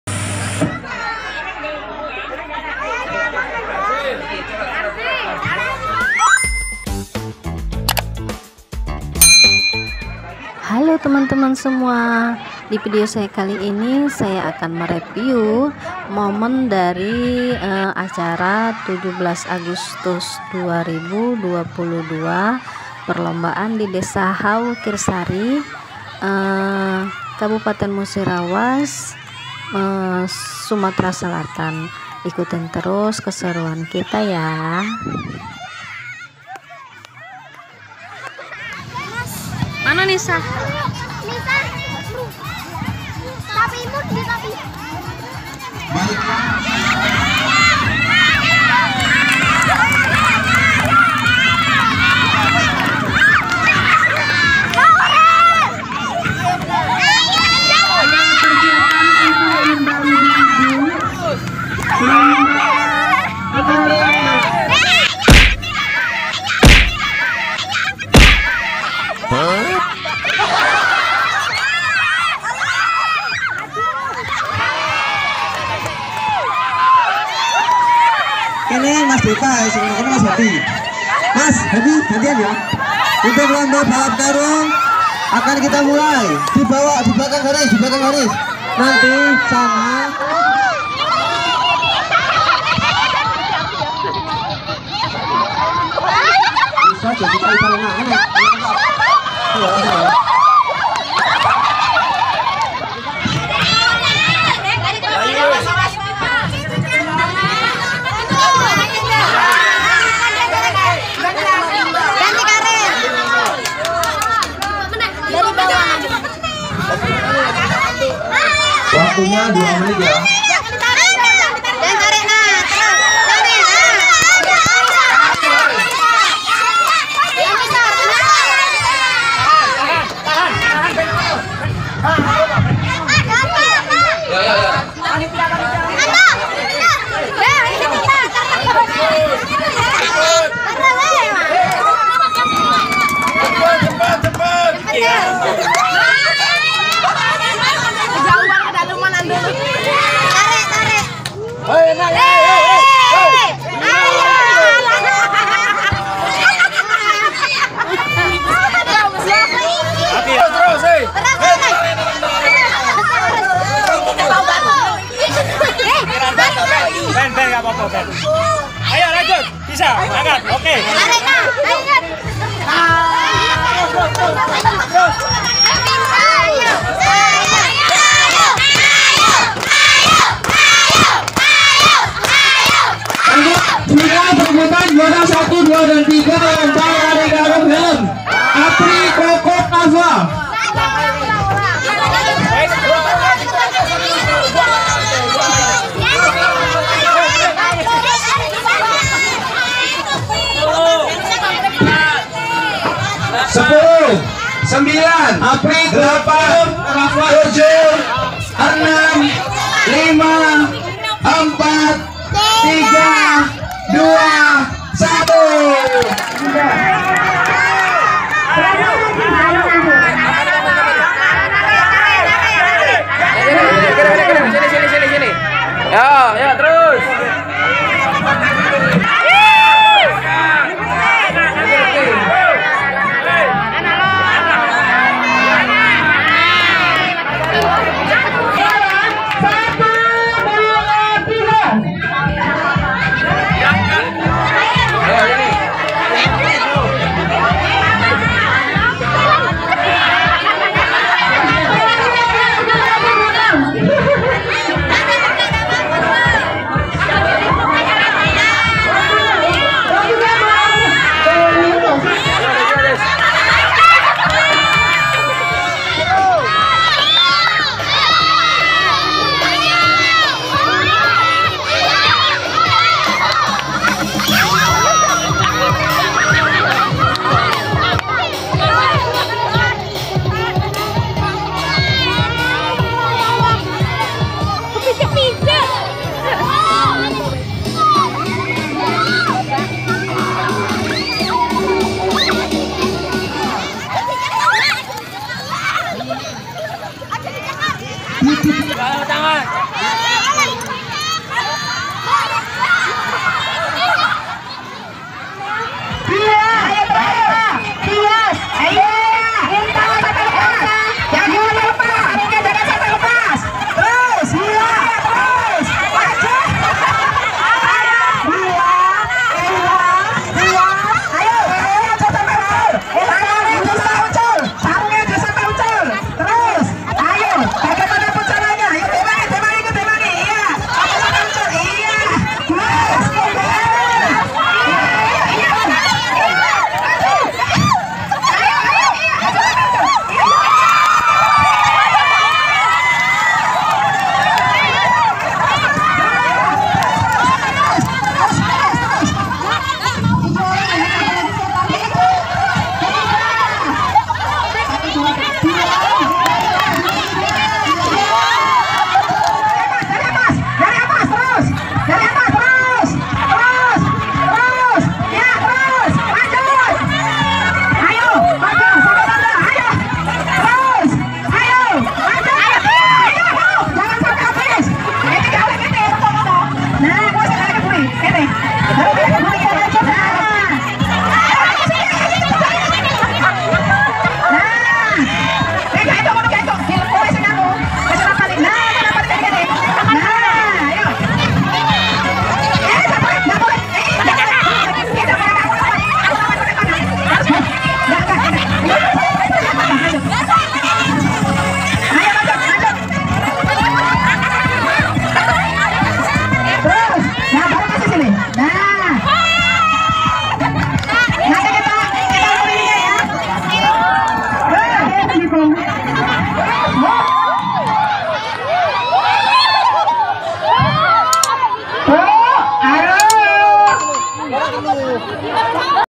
Perlombaan 17 Agustus Di Desa H Wukirsari
Assalamualaikum 🙏🙏
Ikuti keseruan kita di perlombaan 17 Agustus 2022 di Desa H Wukirsari ya teman-teman, Terimakasih 🙏🙏